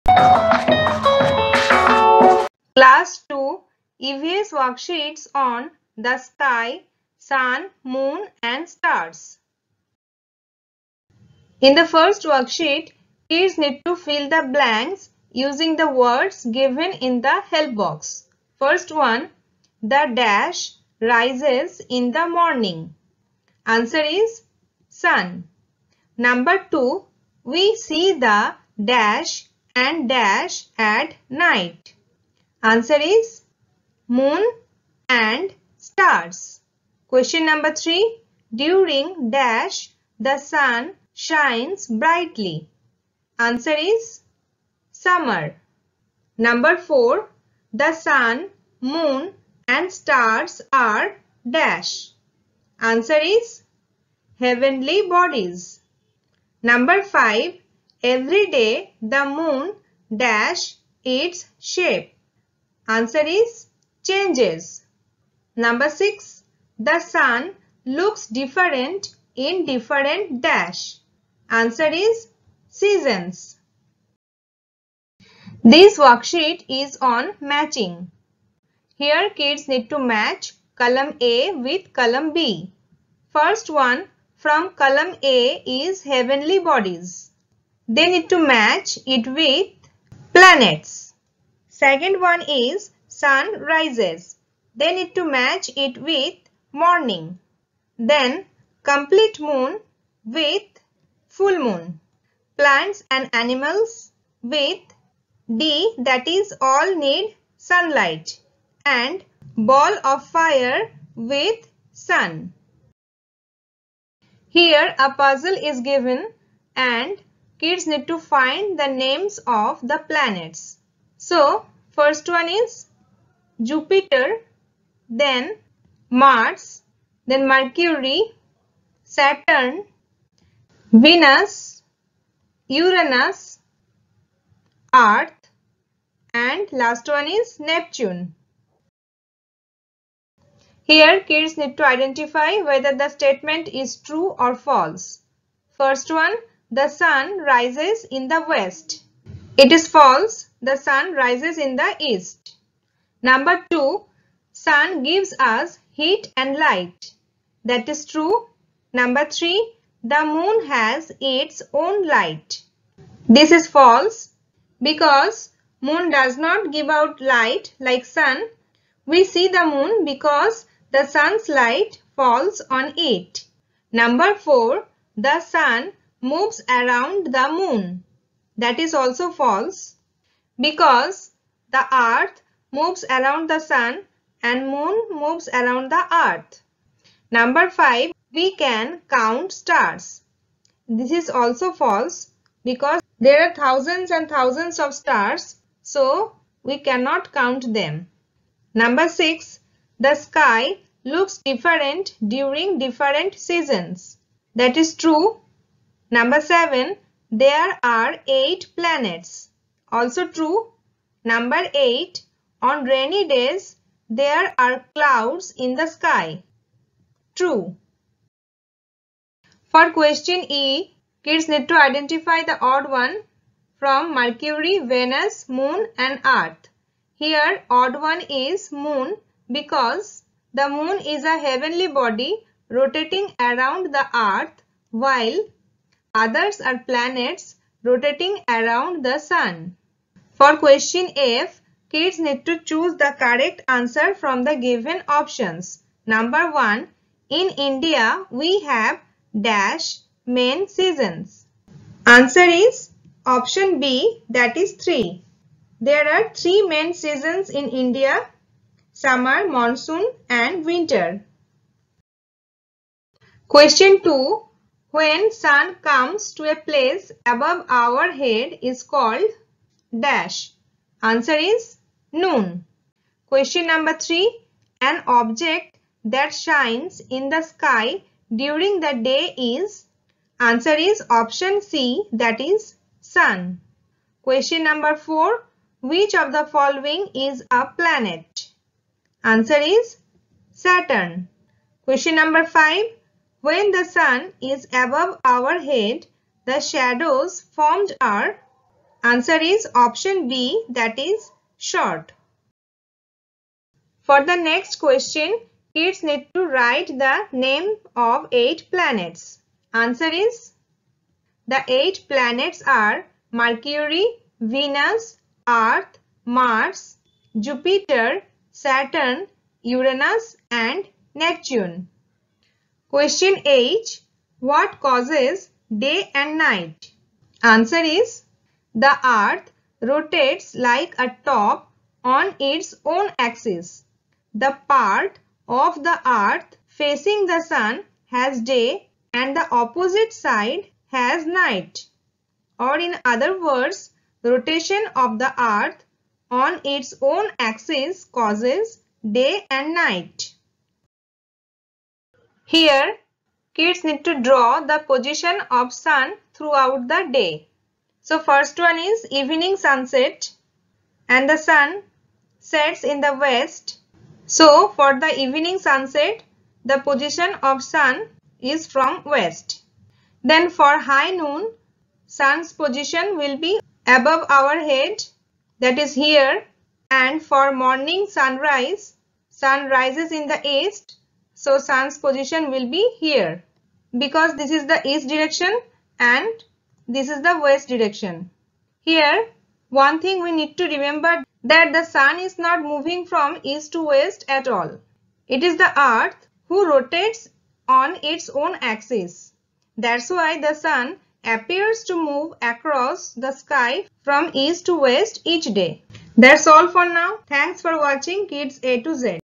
The Sky - Sun, Moon & Stars | Class 2 EVS Worksheet
Today I have shared the EVS worksheet for class 2 students on the topic of the sky - sun, moon, and stars.